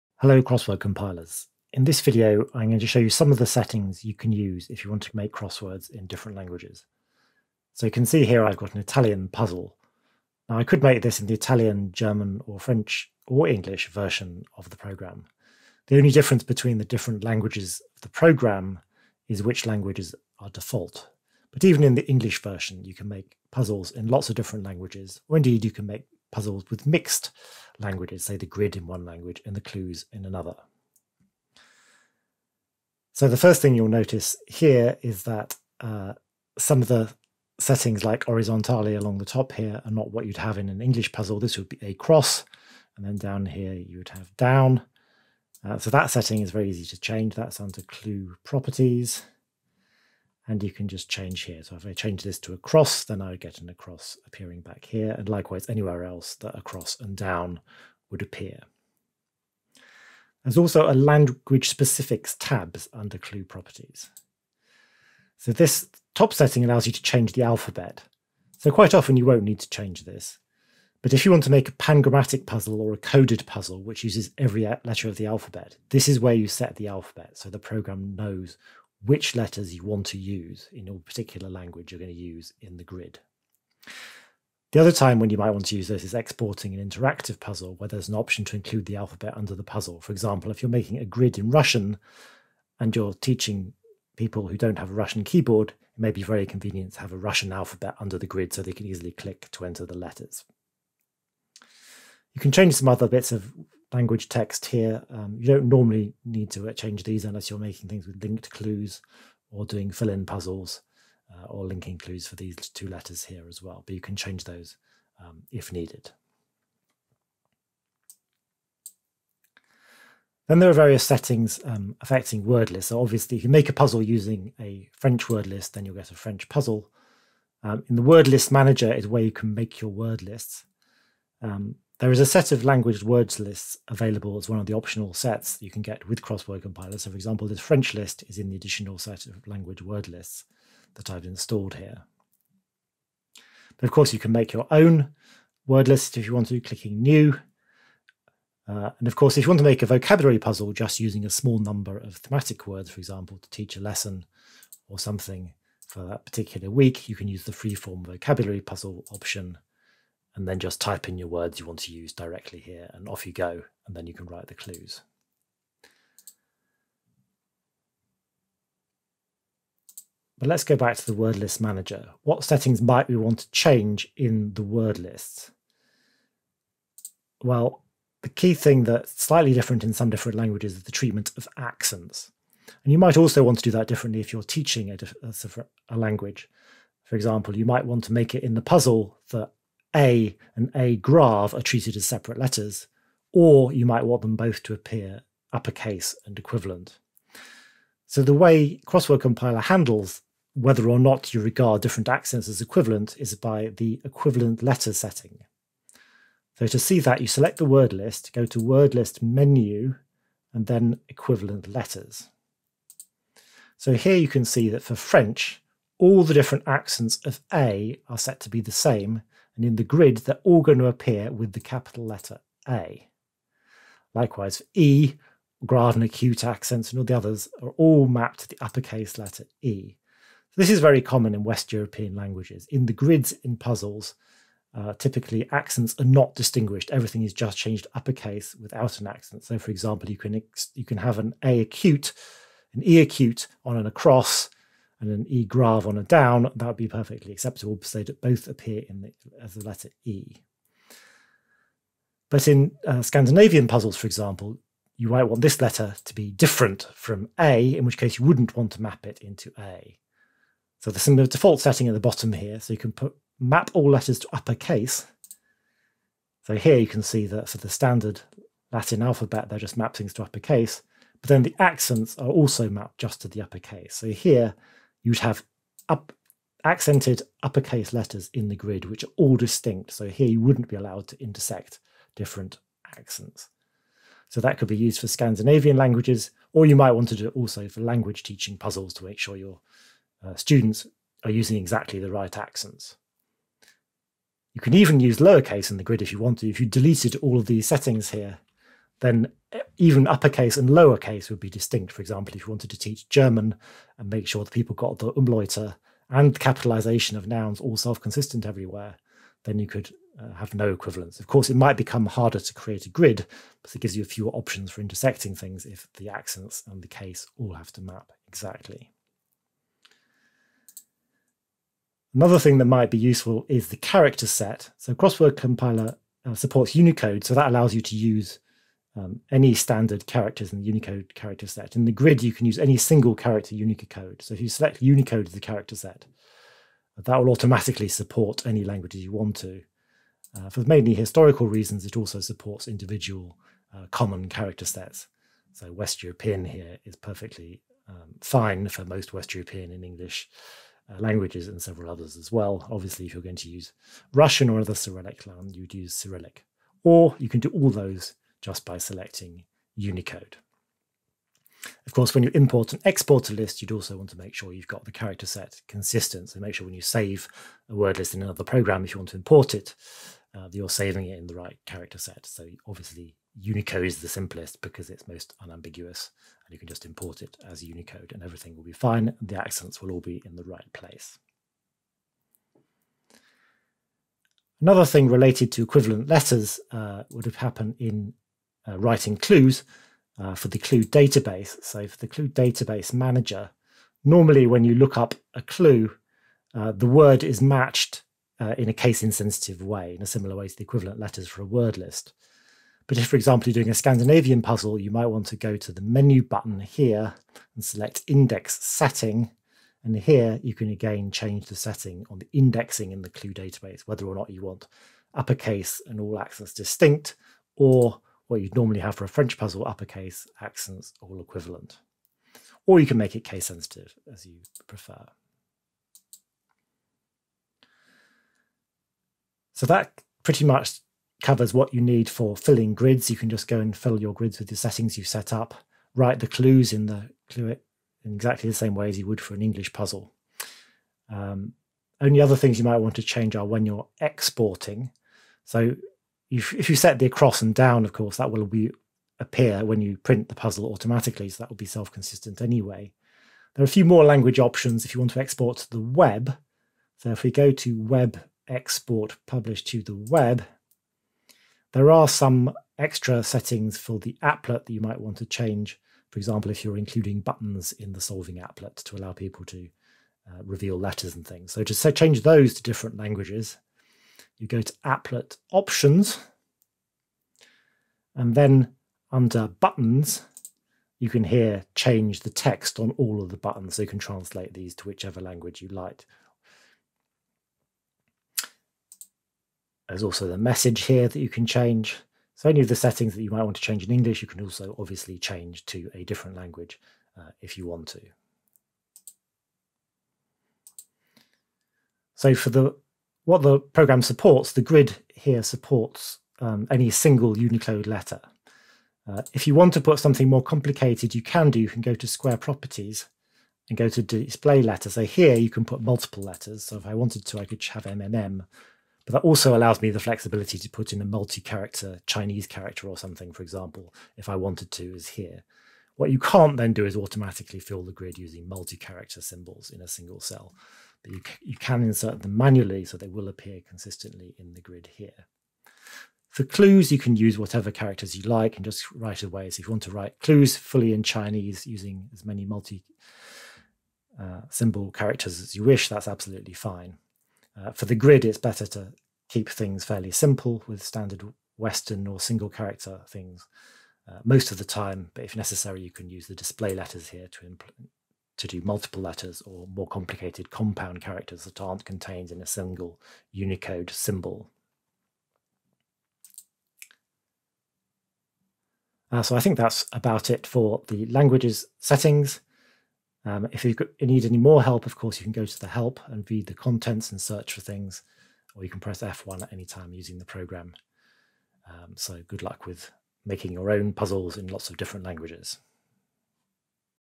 Using different languages
How to make puzzles with different languages for the grid and clues, and change settings for word lists, clue databases and export.